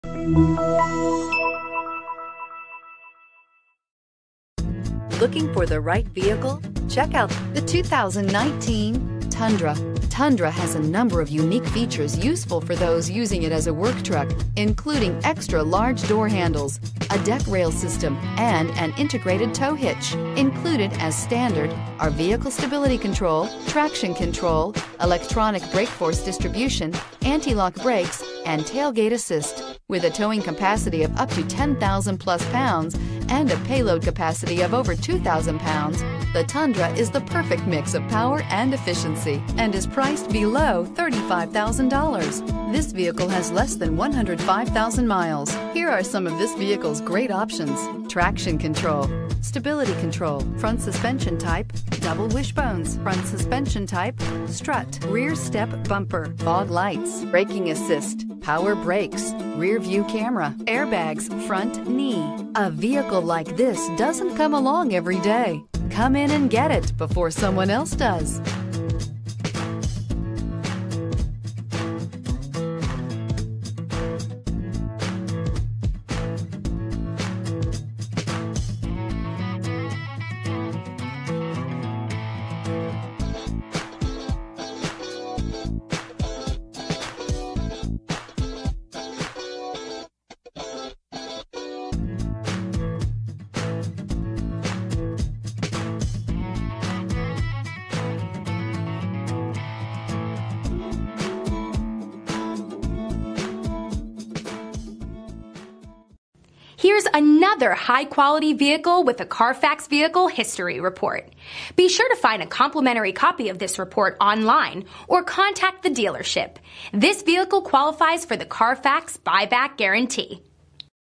2019 Toyota Tundra 1280982A
2019 Toyota Tundra SR5

Auto fans love the backup camera, Bluetooth, a navigation system, braking assist, a power outlet, stability control, traction control, premium sound system, a MP3 player, and anti-lock brakes of this 2019 Toyota Tundra SR5.  We're offering a great deal on this one at $33,630.  This safe and reliable vehicle has a crash test rating of 4 out of 5 stars!  For a good-looking vehicle from the inside out, this vehicle features a sleek magnetic gray metallic exterior along with a black interior.  With touch screen you can safely multi-task from anywhere life takes you.  Do more than just drive: Bluetooth capability provides wireless access for all your electronic devices.  The built-in navigation system means you can explore new neighborhoods without getting lost.  Jam to your favorite tunes with the premium sound system.  Check behind you without twisting your neck using the backup camera.  Interested?